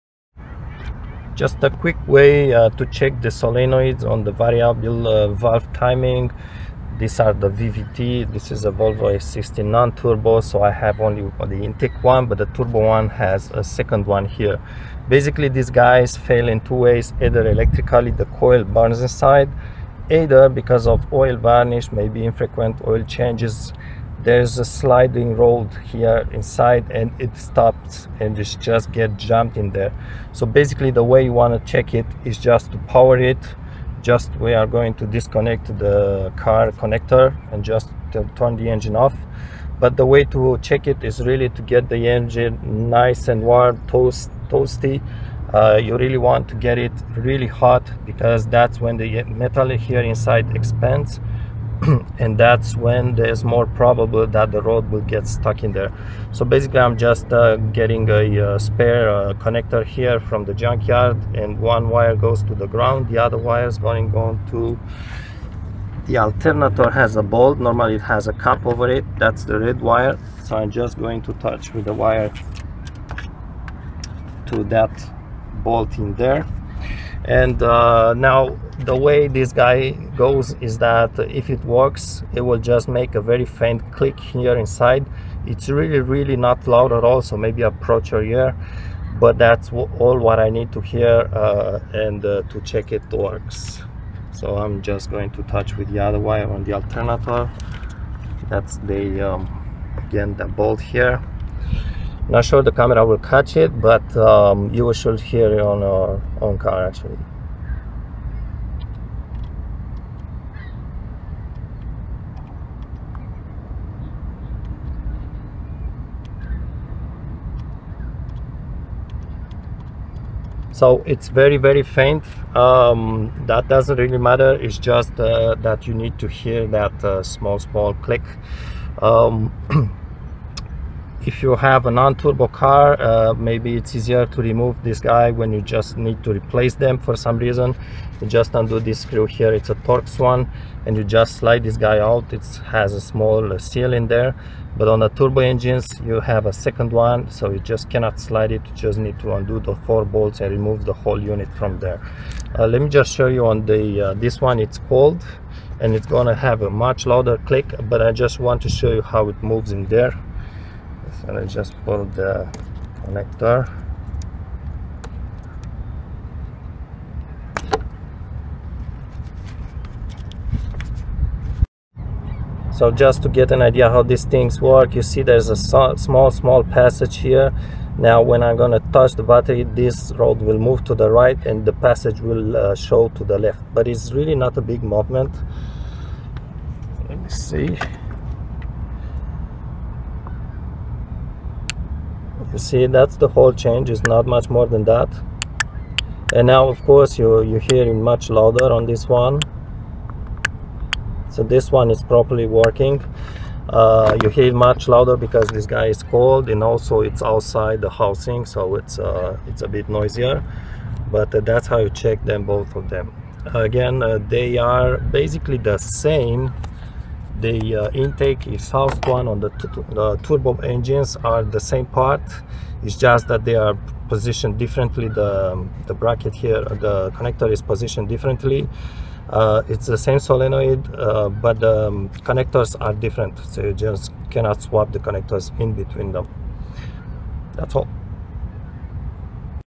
Volvo S60 XC90 V70 Camshaft solenoid (VVT) quick check 2001-2009
Here's how to easily check if the exhaust or the intake VVT solenoids work properly on any of these Volvo engines